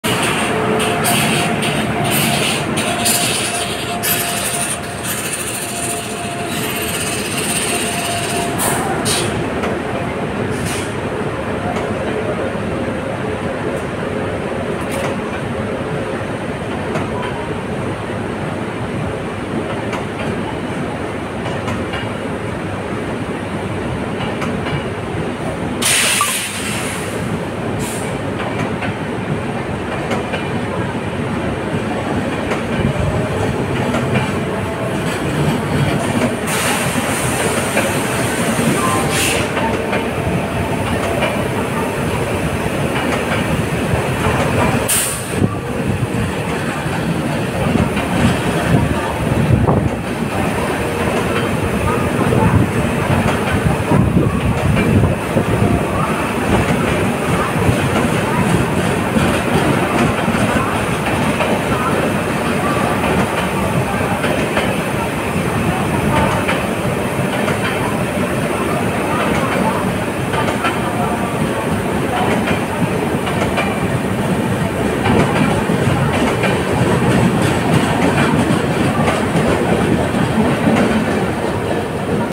Ernakulam To Hazrat Nizamuddin || 12645 Millennium SF Express Train
12645 SF Express Train From Ernakulam To Hazrat Nizamuddin

Ernakulam To Vijayawada Train